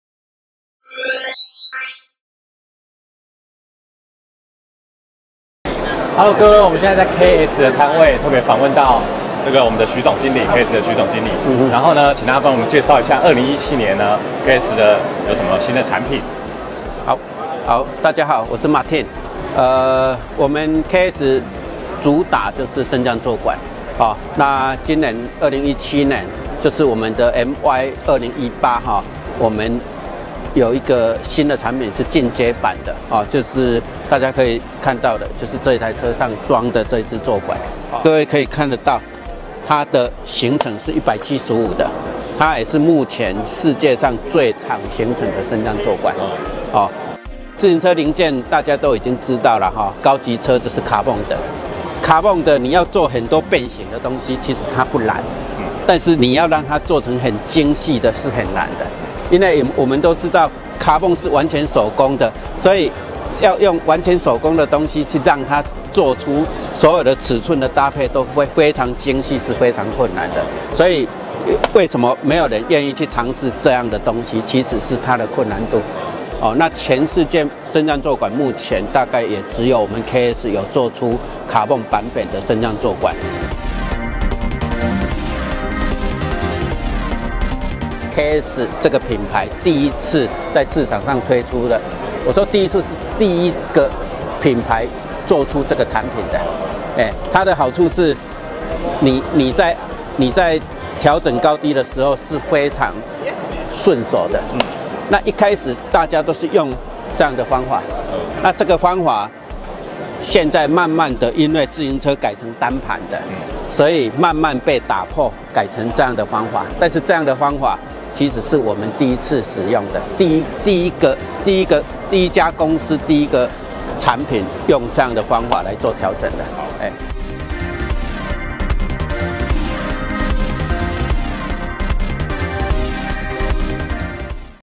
KS凱薩克 LEVCi 伸縮座管簡介
KS凱薩克 LEVCi 伸縮座管，有175mm的伸縮行程，是目前最長的升降座管，除此之外，更特別的是用碳纖維製作而成的呦！😆😆😆